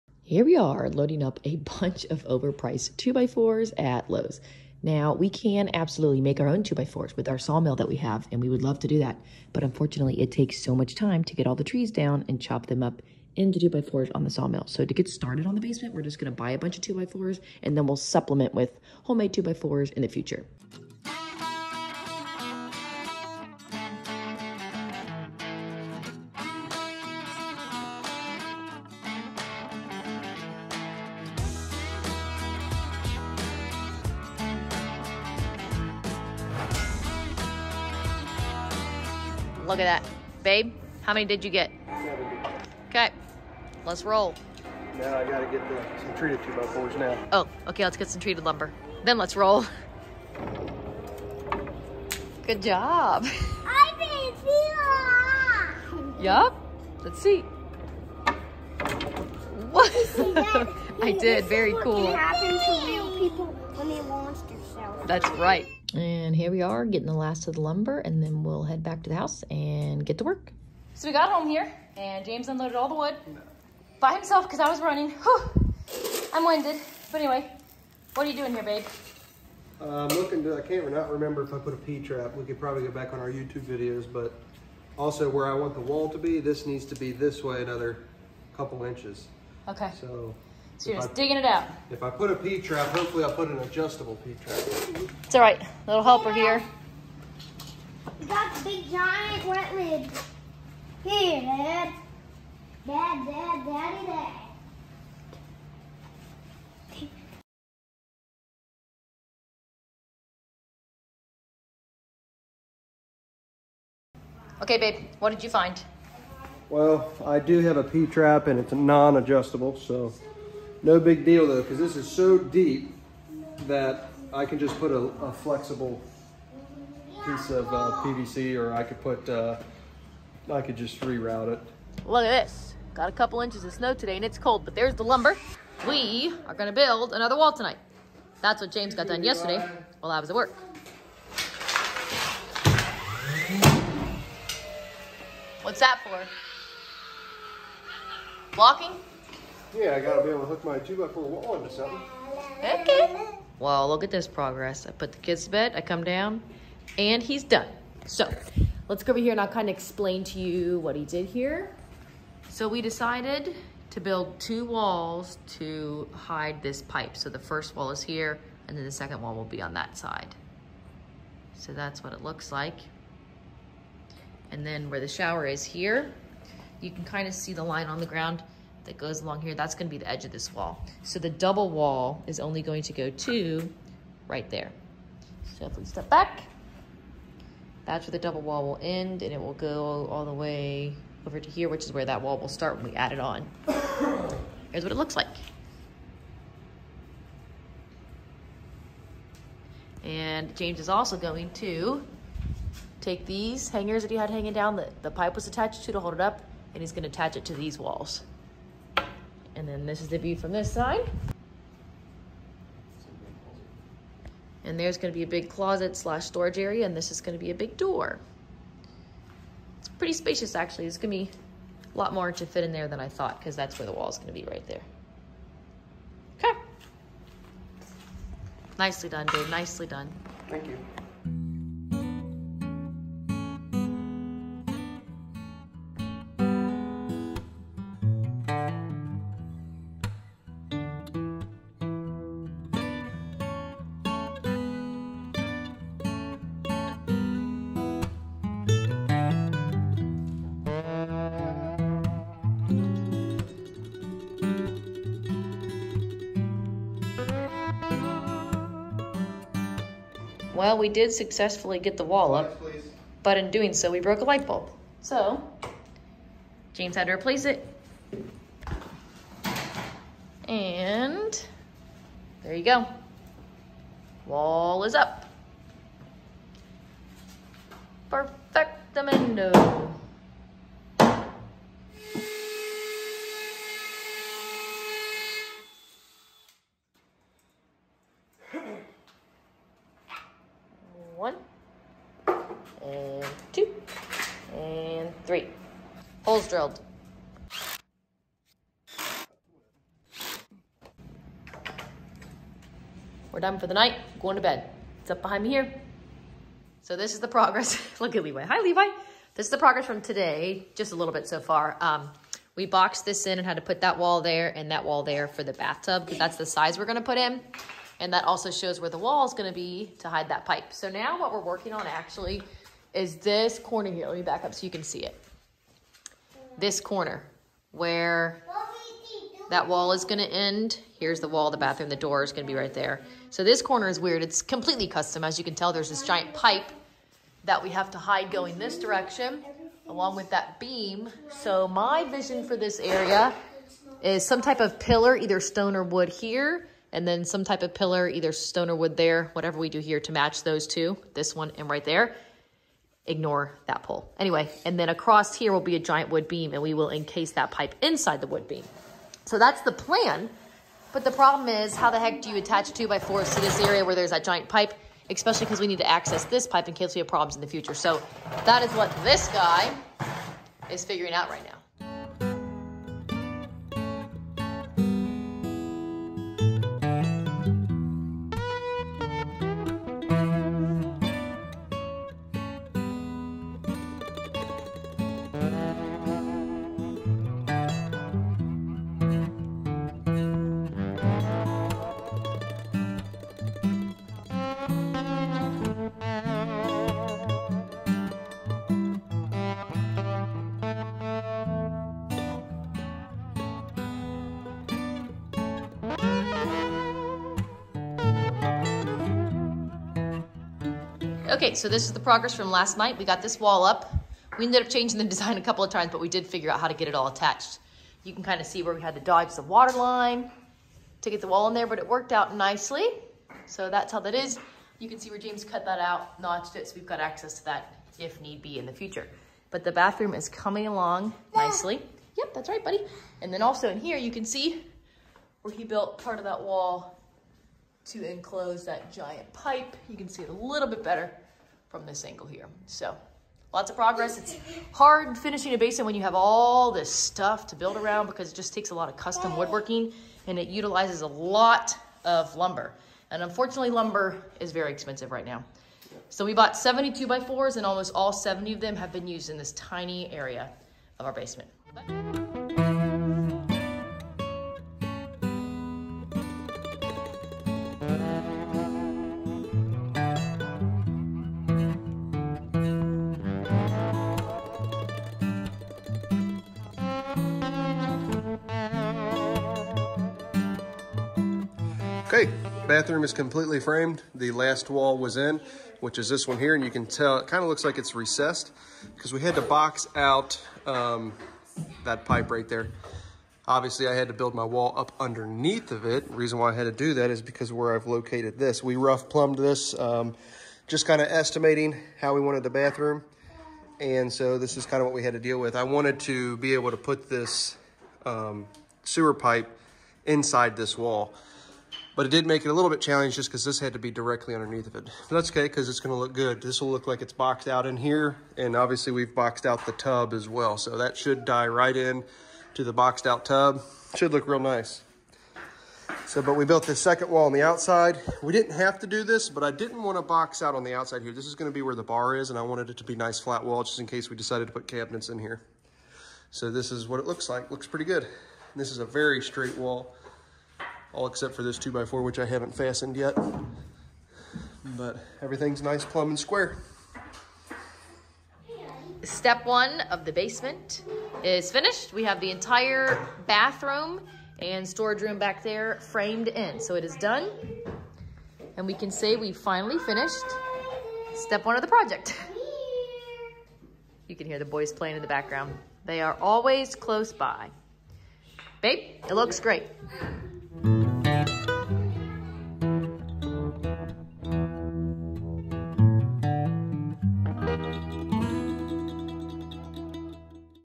Finishing Basement #2: Bathroom Framing (how to hide a 4 inch plumbing pipe in a wall)
In this Collins Country video we start framing in the basement. It was a challenge to enclose and hide the pipe into the wall but we did it. We are learning to be creative with finishing the basement to hide all of the pipes into our walls.